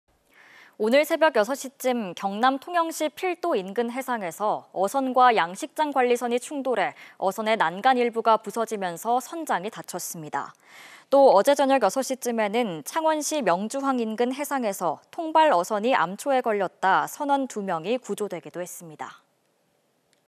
경남 해역에서 선박 사고 잇따라
오늘(12) 새벽 6시쯤 경남 통영시 필도 인근 해상에서 어선과 양식장 관리선이 충돌해 어선의 난간 일부가 부서지면서 선장이 다쳤습니다.

또 어제(11) 저녁 6시쯤에는 창원시 명주항 인근 해상에서 통발 어선이 암초에 걸렸다 선원 2명이 구조되기도 했습니다.